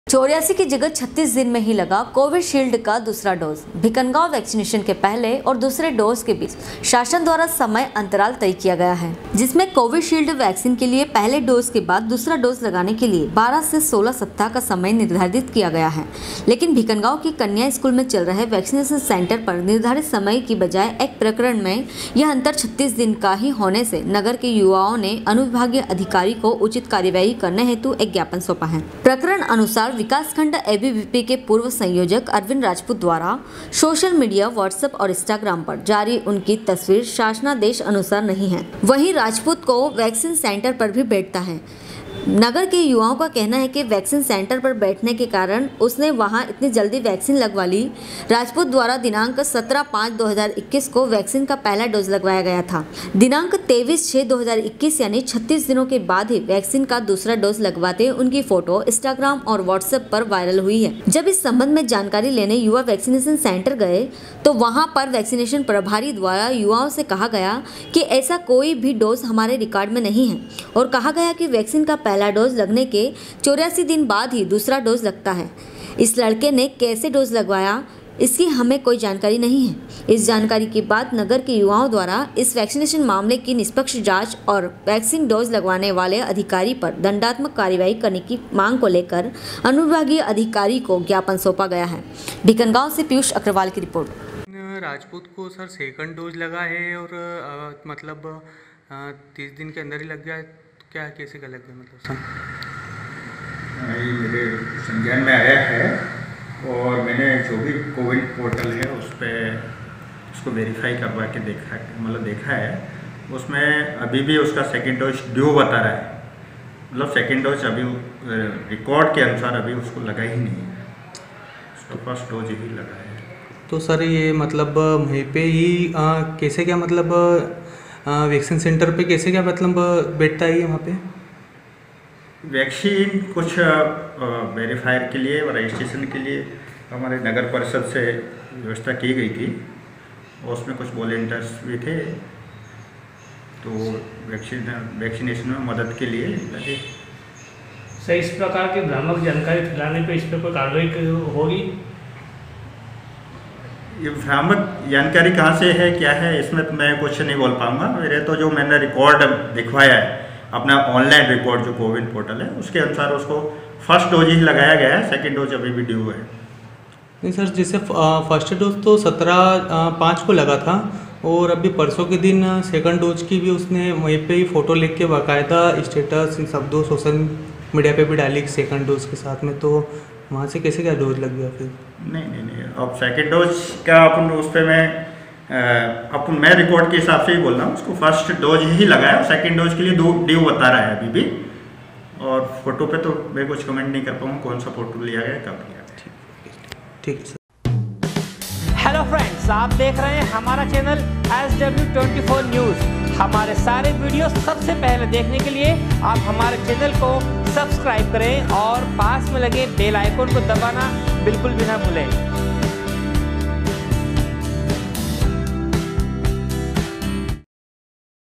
bhikangaon- 84 की जगह 36 दिन में ही लगा कोविशील्ड का दूसरा डोज भीकनगांव वैक्सीनेशन
84 की जगह 36 दिन में ही लगा कोविशील्ड का दूसरा डोज भीकनगांव वैक्सीनेशन के पहले और दूसरे डोज के बीच शासन द्वारा समय अंतराल तय किया गया है। भिकनगाॅव से पियूष अग्रवाल की रिर्पोट।